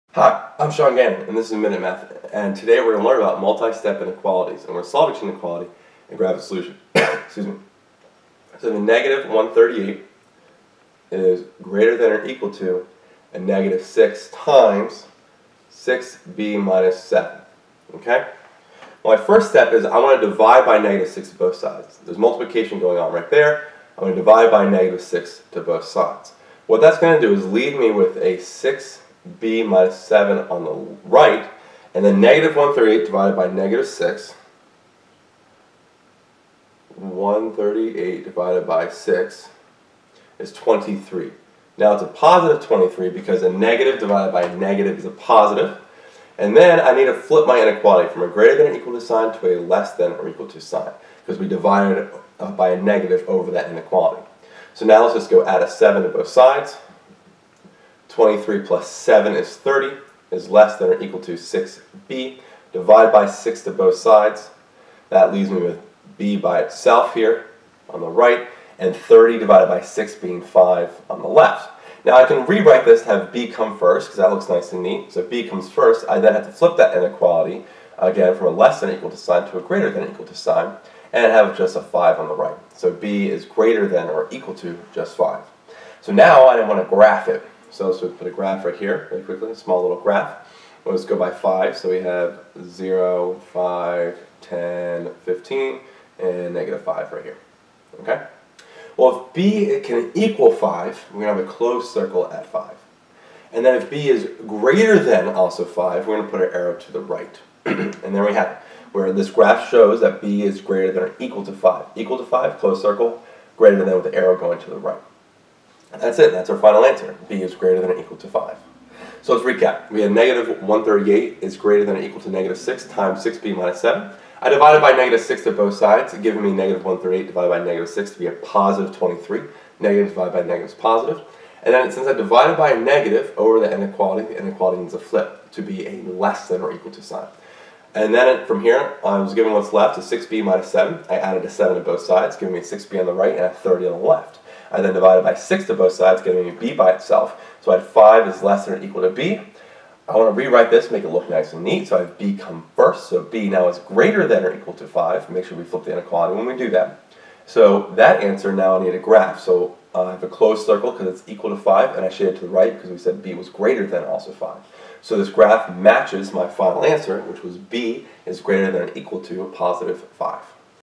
Solve and graph -138≥-6(6b-7)
In this math video lesson on Multi-Step Inequalities, I solve -138 greater than or equal to -6(6b-7) and graph its solution.